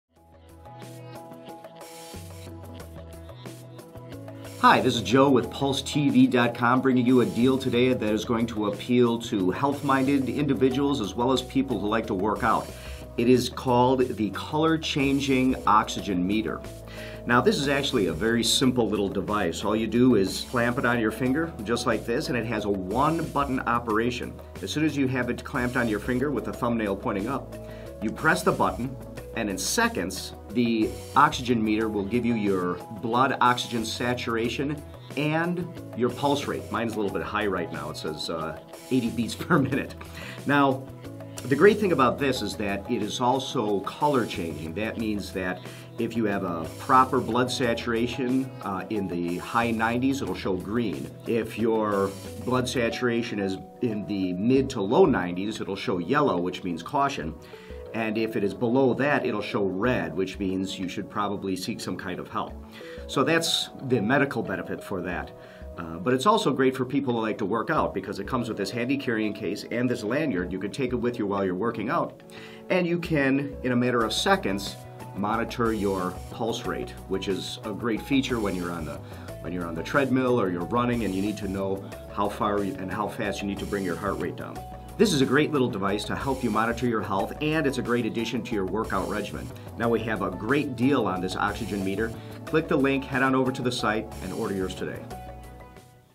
Color Changing Oxygen Meter
The Color Changing Fingertip Oxygen Meter is a portable, non-invasive, spot-check for blood oxygen saturation (SpO2) and pulse rate. The Meter requires no routine calibration and operation is simple and convenient. The product is small and light weight making it easy to carry with you.